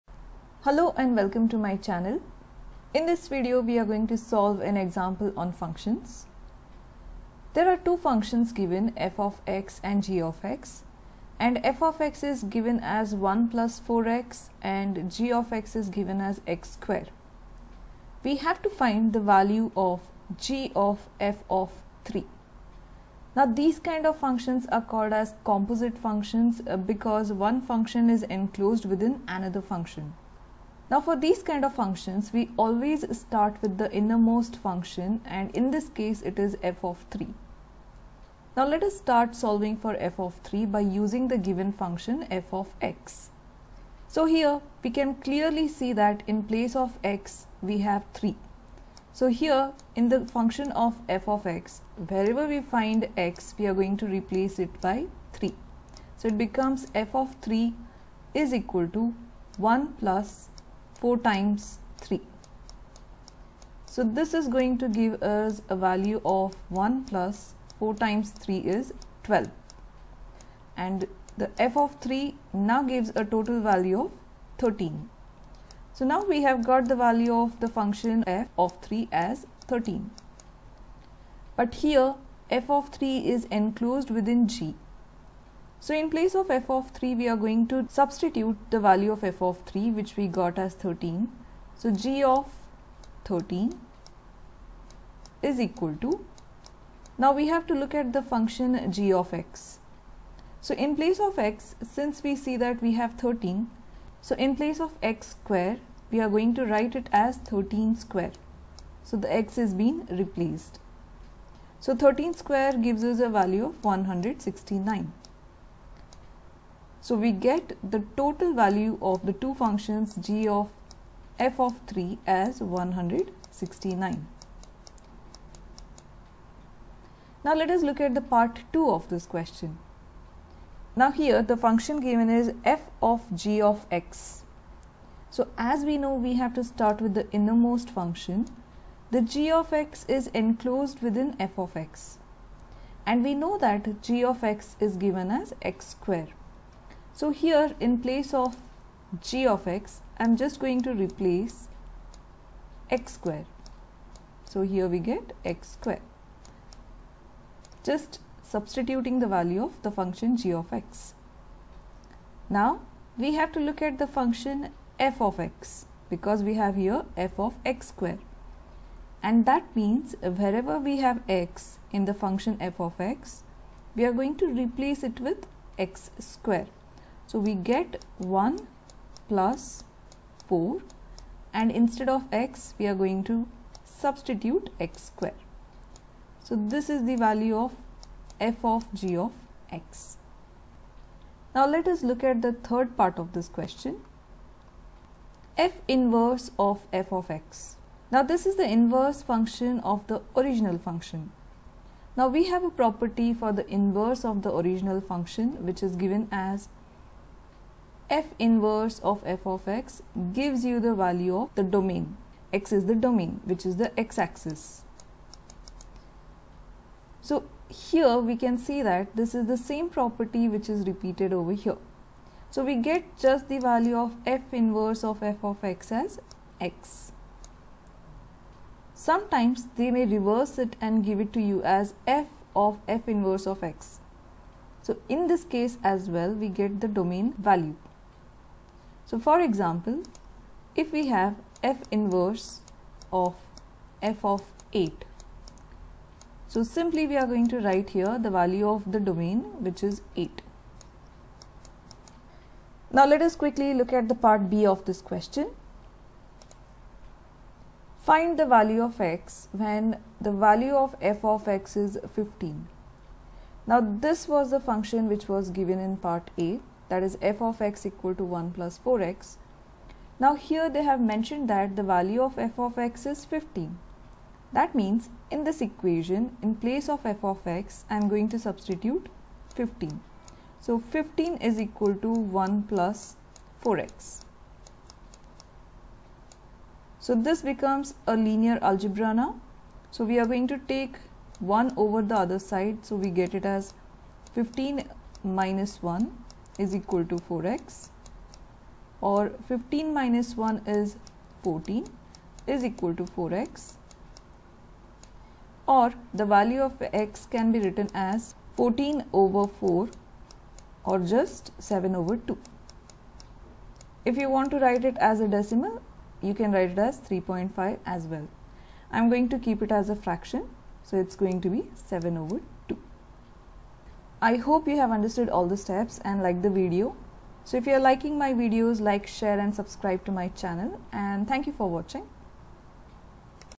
If f(x) = 1+4x and g(x) = x², find the given functions.
The video explains a way to solve an example on composite functions using the properties of functions and composite functions.

If f(x) = 1+4x and g(x) = x², find gf(3), fg(x), f inverse of f(x).